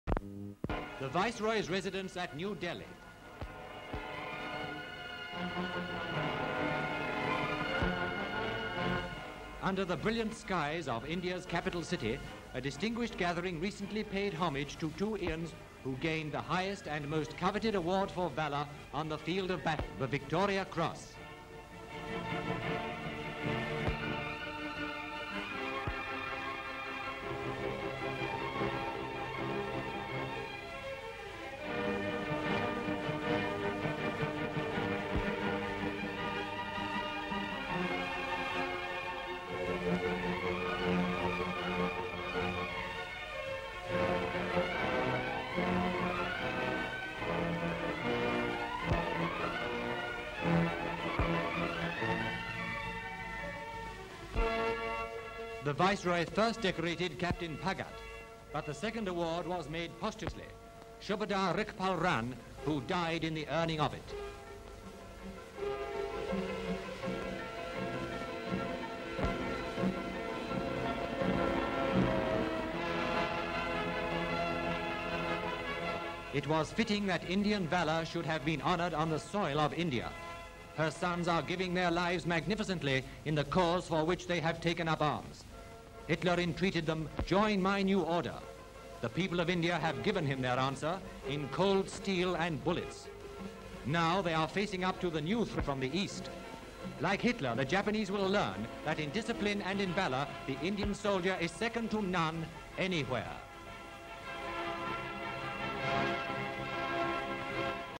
INDIA / DEFENCE: World War II: Indian war heroes honoured with Victorian Cross (1942)
Two Indian war heroes - Captain Paghat and Shubadar Richpal Ran (now dead) - are honoured for their bravery in war when presented with the Victoria Cross by the Viceroy of India in a ceremony in New Delhi.

Full Description:

INDIA: New Delhi:
EXT
Exterior shot Viceroy's Residence
GVs crowd who have come pay homage to two Indians who are to be decorated
Indians seated wearing turbans
Close up Victoria Cross
Indian soldier shoulders arms
Viceroy of India (Marquess of Linlithgow) and Vicereine walking towards camera
Two Indian military officers towards camera and stand before Viceroy - they salute him and shake hands one by one
Close up Indian wearing turban
Viceroy decorates Captain Paghat with Victoria Cross
Close up Victoria cross on Paghat's tunic
Paghat salutes Viceroy
Viceroy takes off his hat to relatives of second "VC" who died earning it
Still photograph of the second "VC" - Shubadar Richpal Ran
Wide shot of ceremony
Viceroy gives medal to veiled woman and shakes hands with her
Viceroy - crowd behind and he walks through them
Viceroy and party up gangway made by people
Troops in full kit marching away from camera - pyramids in foreground
Side view Indian band past camera
Indian troops wearing turbans marching towards camera
Background: Two Indian war heroes - Captain Paghat and Shubadar Richpal Ran (now dead) - are honoured for their bravery in war when presented with the Victoria Cross by the Viceroy of India in a ceremony in New Delhi.